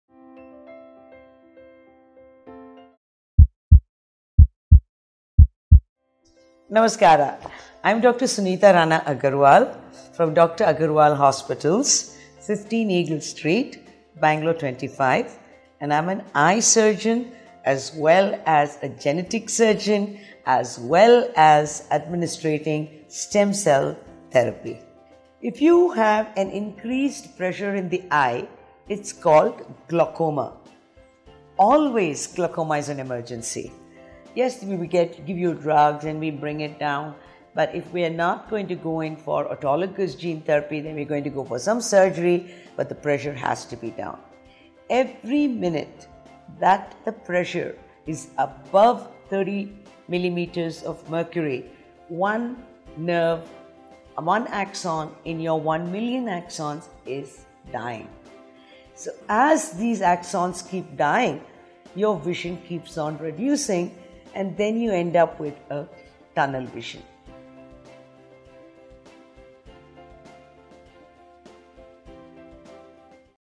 Why do I feel pressure in my eyes?Is it an emergency? - Dr. Sunita Rana Agarwal
If you have an increased pressure in the eye, it is called as glaucoma. Always  glaucoma is  an emergency, we  can give you drugs ad  Brigit down but if you are not going to go in for autologus gene therapy, then  we are  going to go for  some  surgery then the pressure has to be down. Every minute that the pressure is above 30 mm to mercury one axon in your 1  million axon is  dying.  As these axons keep dying, your vision keeps on reducing and you end  up with a tunnel vision.